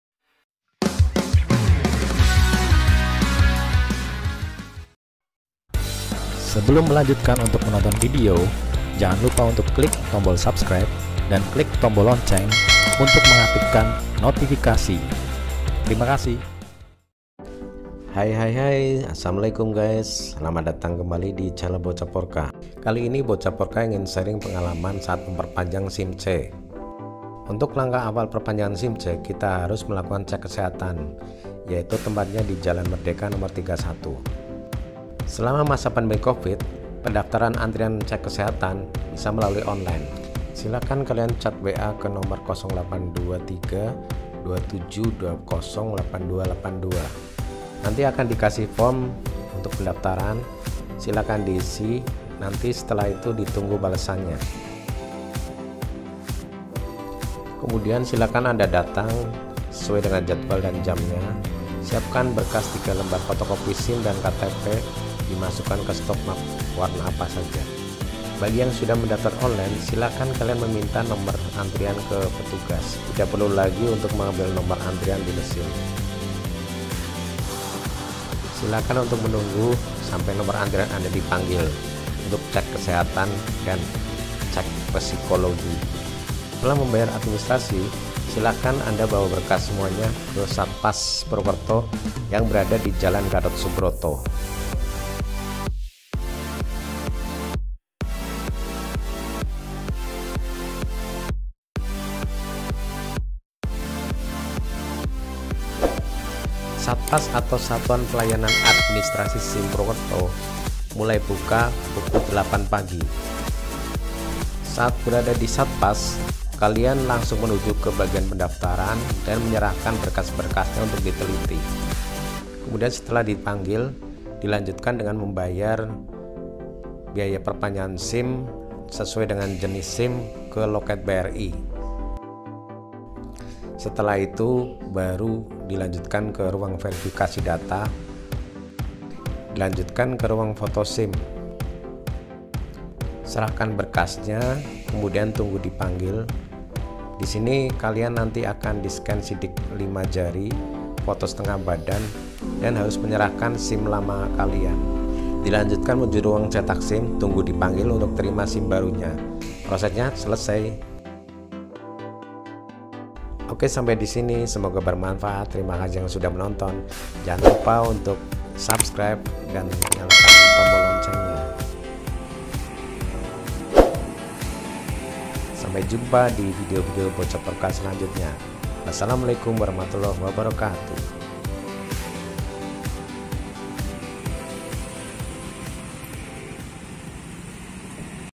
PROSES PERPANJANGAN SIM C DI SATPAS PURWOKERTO TAHUN 2020
Proses perpanjangan SIM C di SATPAS ( Satuan Penyelenggara Administrasi SIM di kota Purwokerto tahun 2020 saat pandemi COVID19